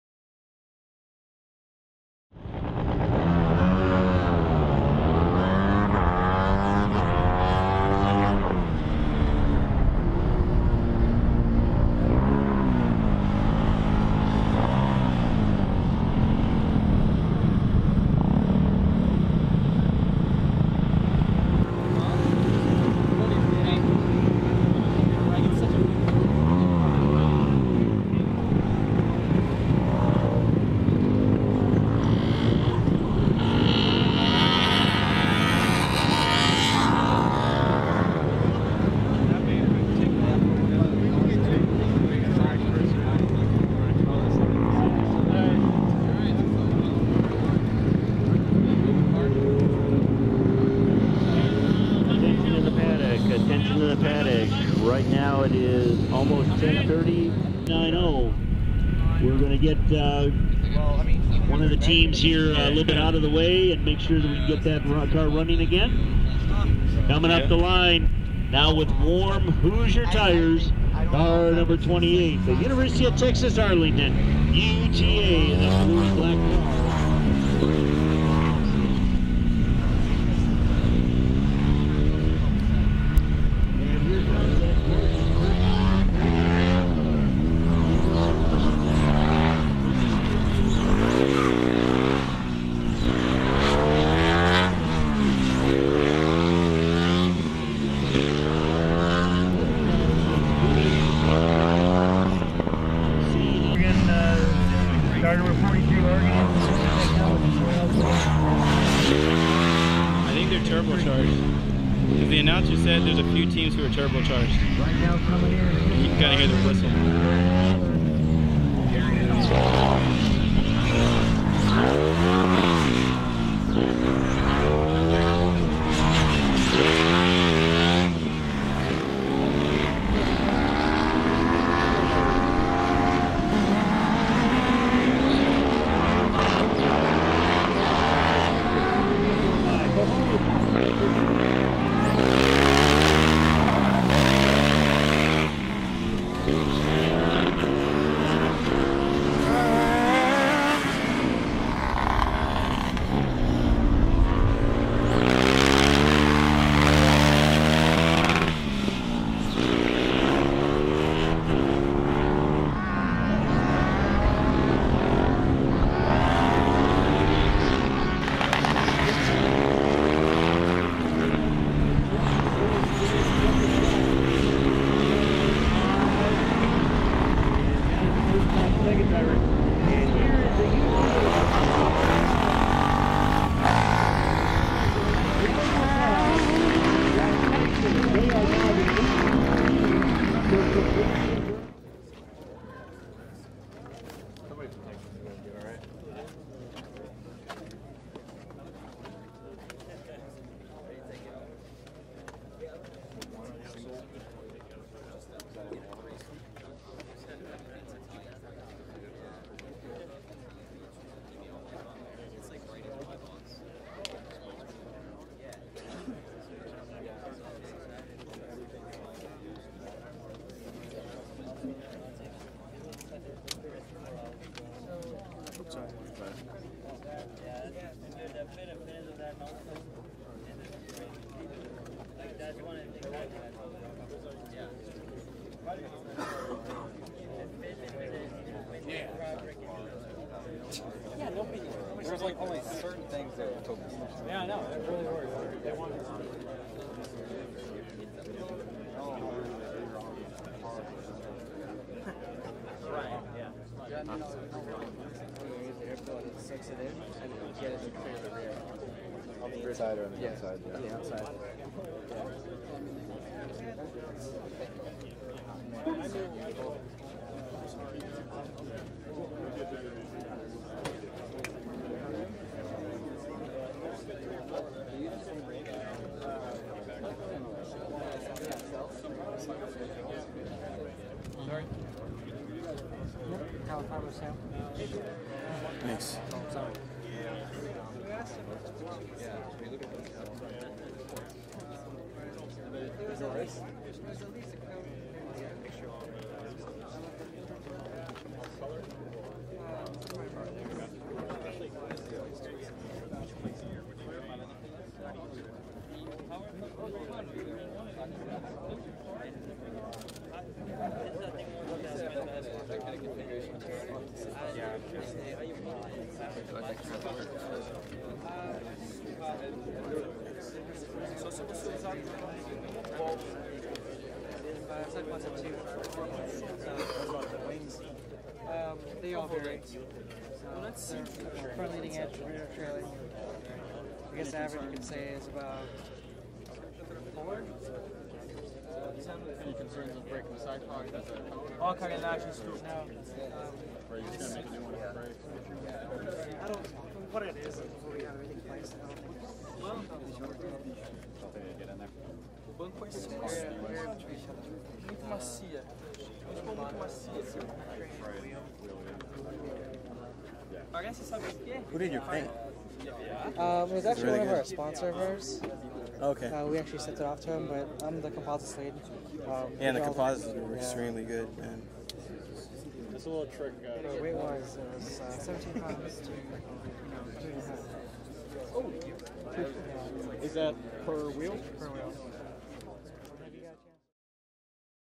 University of Texas Arlington Formula SAE West 2012 | Walk Around, Acceleration, & More
University of Texas at Arlington. 3rd place in design, 2nd place overall at FSAE West 2012 in Lincoln, Nebraska.

Formula SAE is a student design competition organized by the Society of Automotive Engineers (SAE, also known as SAE International). The concept behind Formula SAE is that a fictional manufacturing company has contracted a student design team to develop a small Formula-style race car using a 610cc or under motor. The prototype race car is to be evaluated for its potential as a production item. The target marketing group for the race car is the non-professional weekend autocross racer. Each student team designs, builds and tests a prototype based on a series of rules, whose purpose is both ensuring on-track safety (the cars are driven by the students themselves) and promoting clever problem solving. The prototype race car is judged in a number of different events.